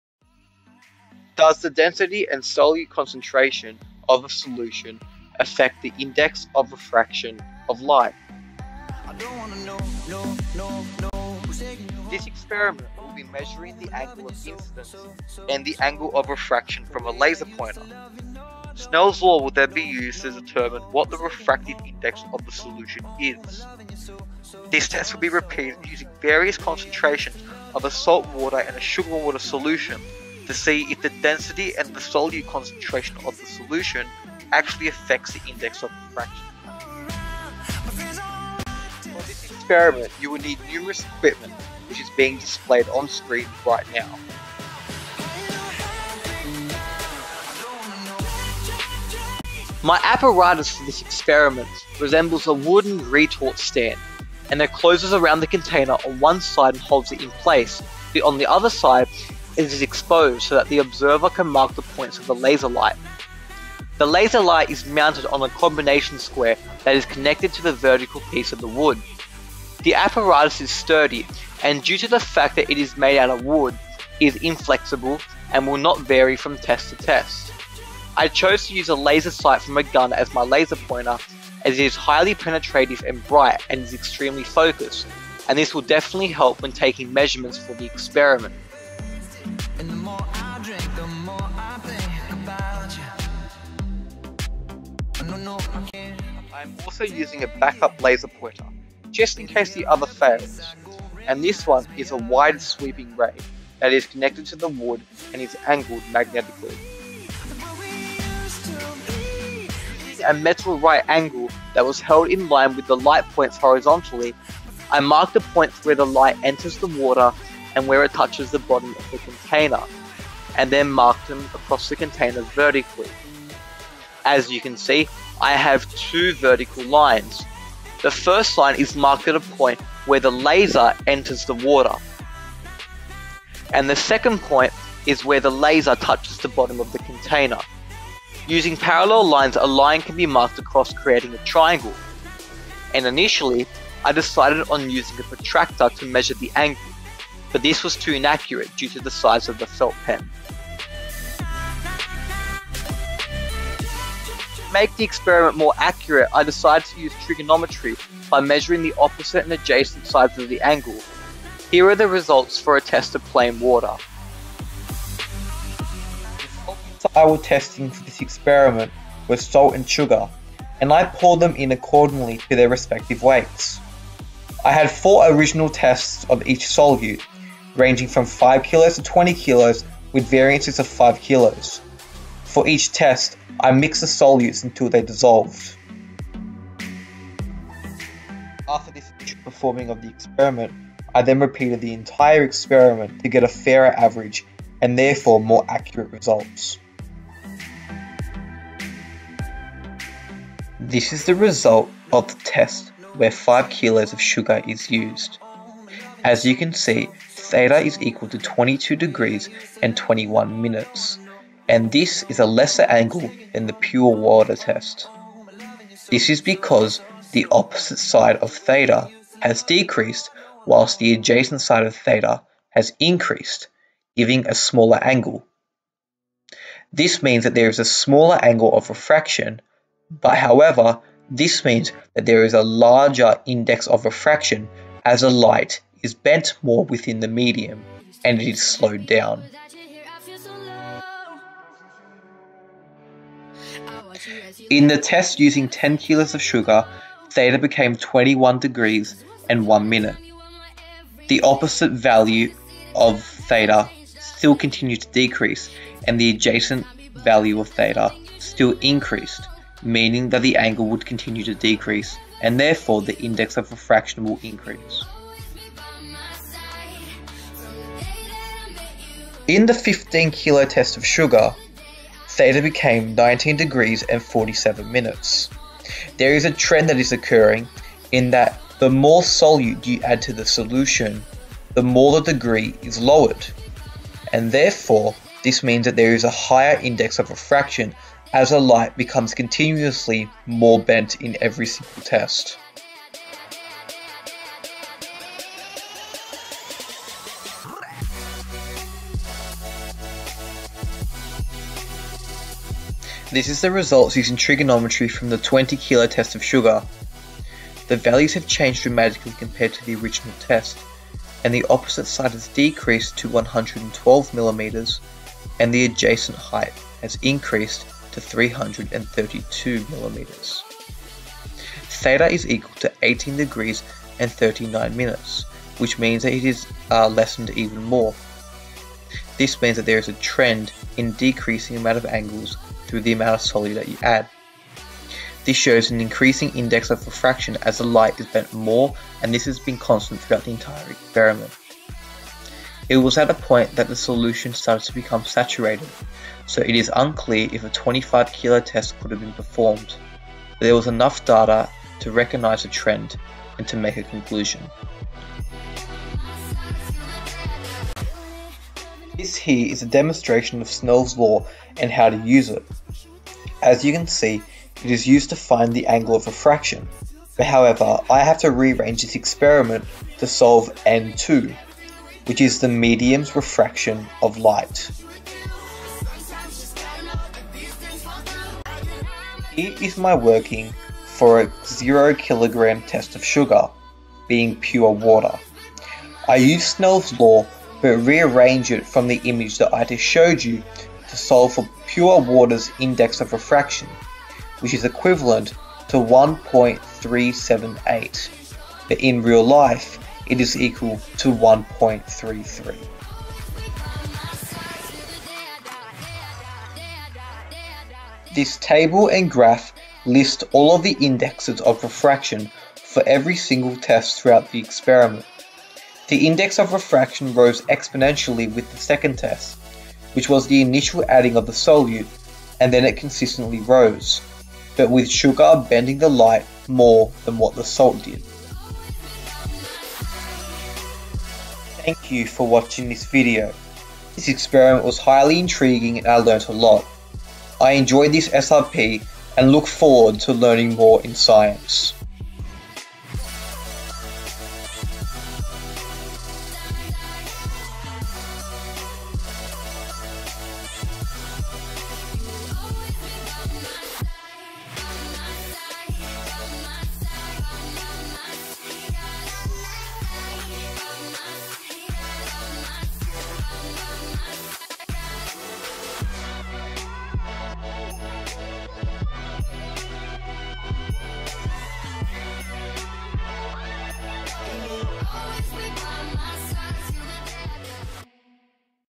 Year 10 - SRP - Index of Refraction
Year 10 Science SRP - A Study of the Refraction of Light and how it changes when the solute concentration is adjusted.

By Alessandro Comin